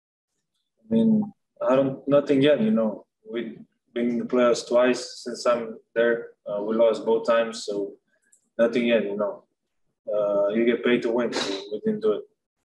La dura autocrítica de Luka Doncic: "Todavía no demostré nada, me pagan por ganar".
El base de Dallas Mavericks expresó su enojo tras la eliminación a manos de Los Angeles Clippers en el séptimo partido de la serie de primera ronda de los playoffs de la NBA.